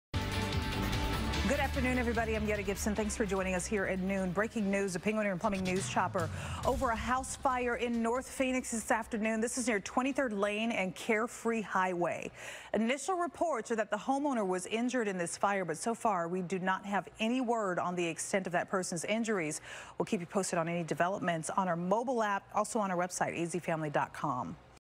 First-alarm fire scorches house near Anthem; homeowner injured
A first-alarm fire scorched a home near Anthem Thursday.